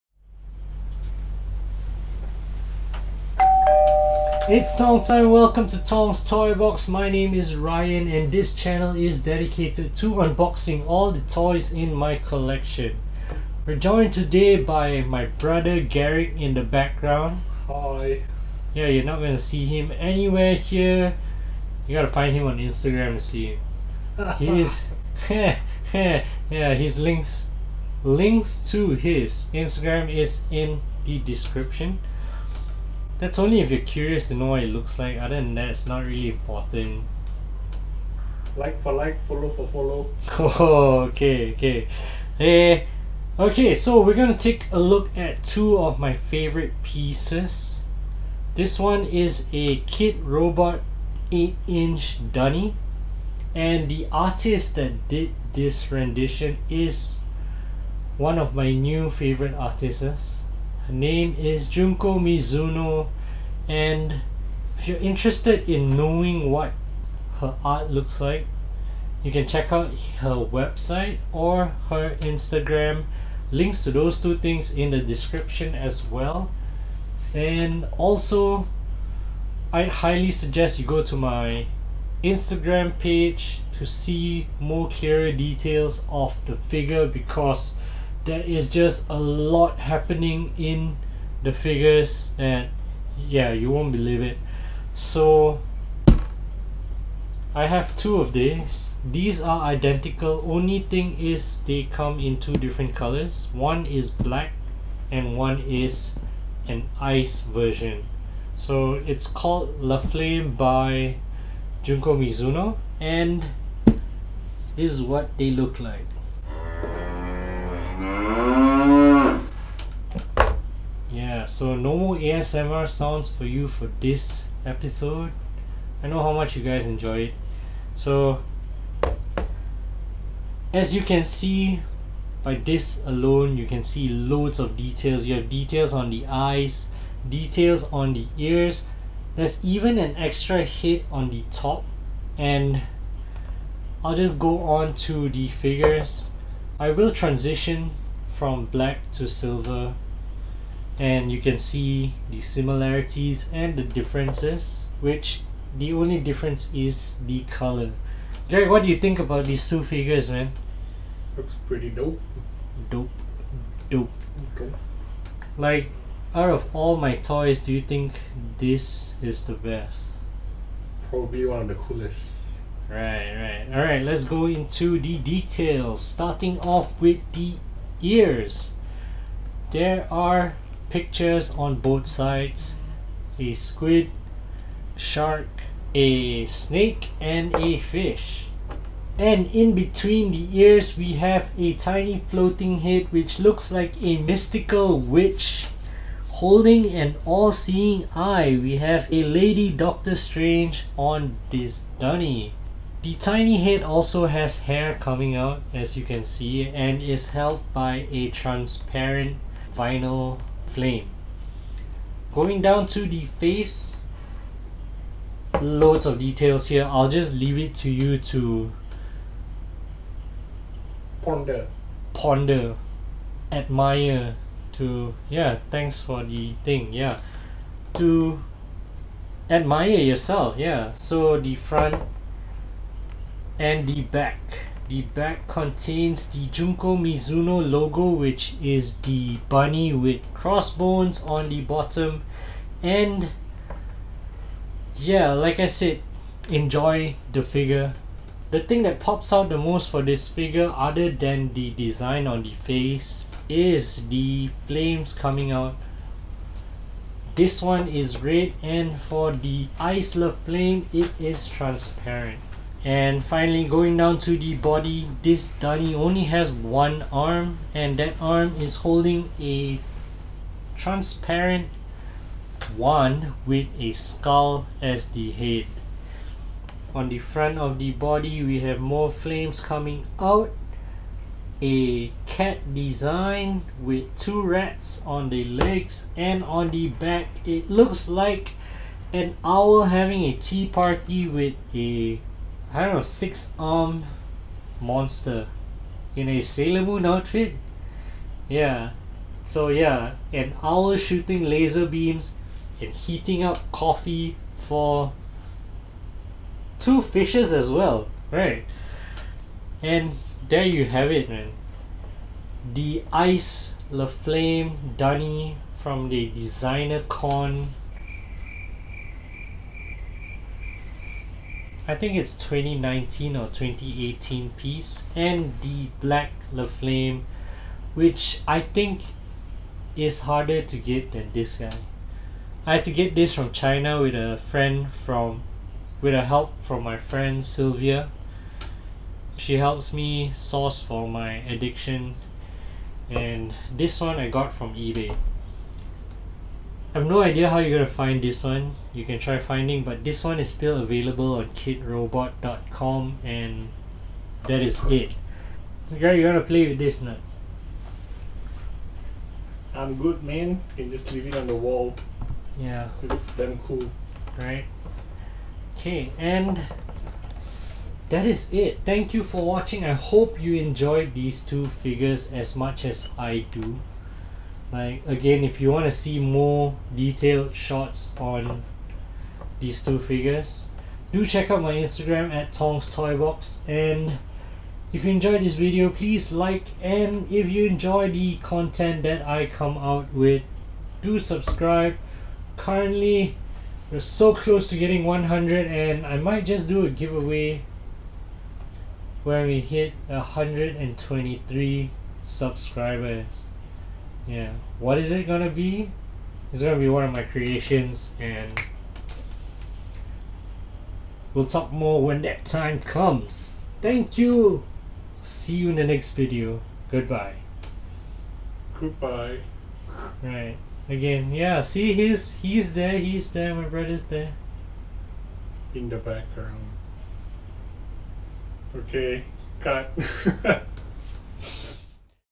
Kidrobot × Junko Mizuno's Black La Flamme and DCON Ice La Flamme (2020 Unboxing Review)
Green Edition La Flamme Dunny (If you're in Malaysia)

A little closeups on my 2 favourite Dunnies. Let's check out the details. We're getting full on 360° views on each of these dunnies so let's take a look at them together.

Background guy a.k.a. my brother/my google when I'm stuck with editing